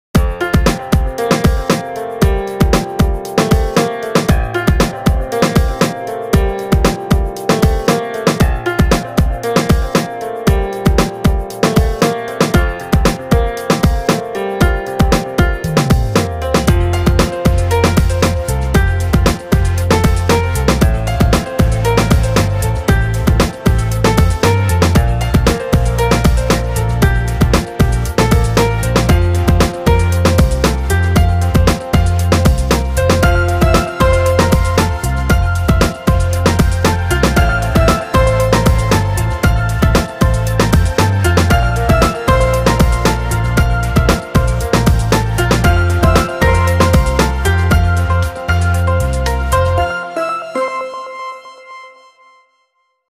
Covid-19 Test at Hongkong Asia World Expo Part 2 (May2020)
this video part 2, process at Asia Expo for the saliva test.